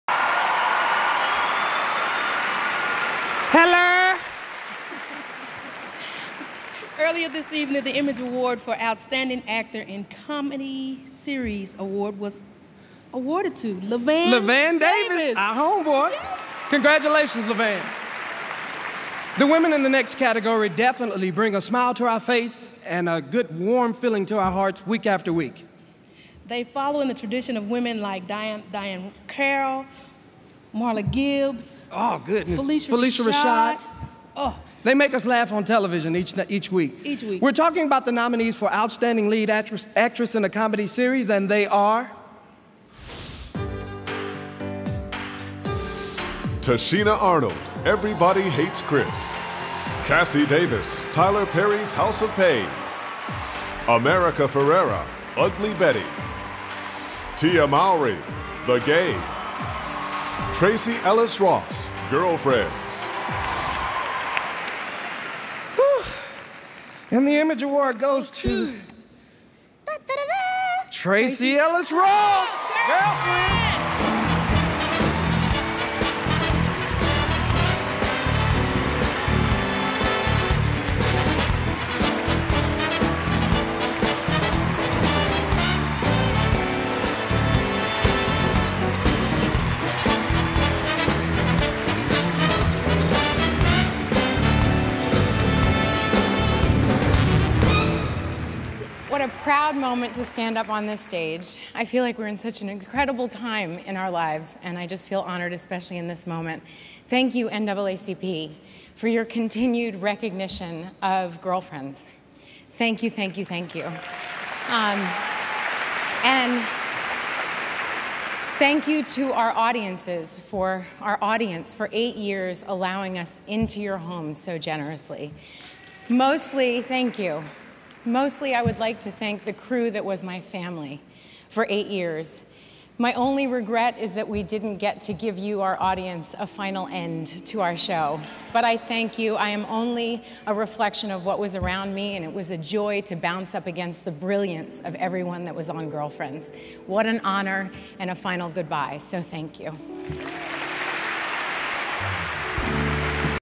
Tracee Ellis Ross - 40th NAACP Image Awards - Outstanding Actress in a Comedy Series
Tracee Ellis Ross receives the award for Outstanding Actress in a Comedy Series at the 40th NAACP Image Awards